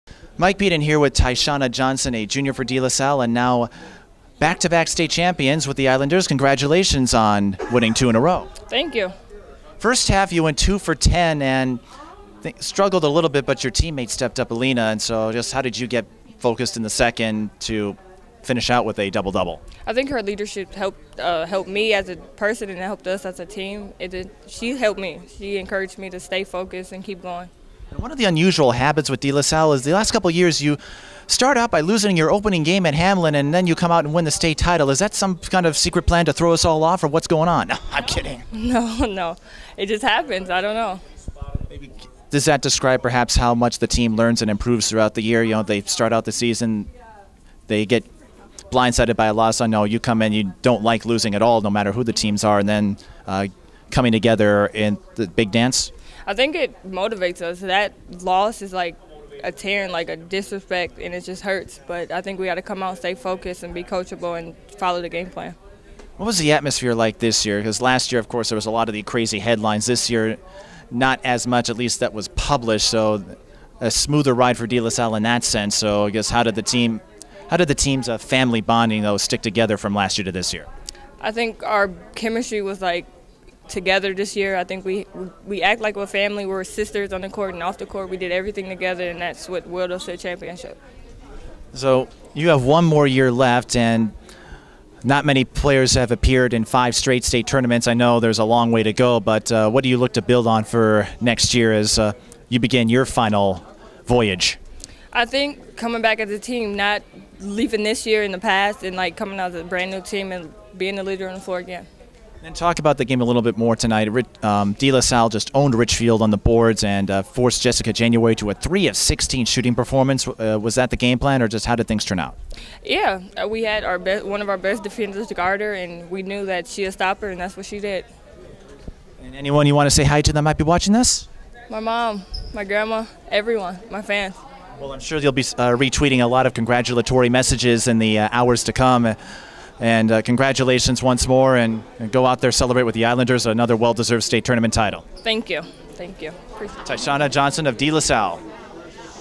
2012 state tournament - DeLaSalle forward Tyseanna Johnson
DeLaSalle forward Tyseanna Johnson, along with guard Allina Starr, have made four straight state tournament appearances and now have two trophies to their credit. The undersized post plays much bigger than her height would suggest, finishing with a double-double in the class AAA final against Richfield.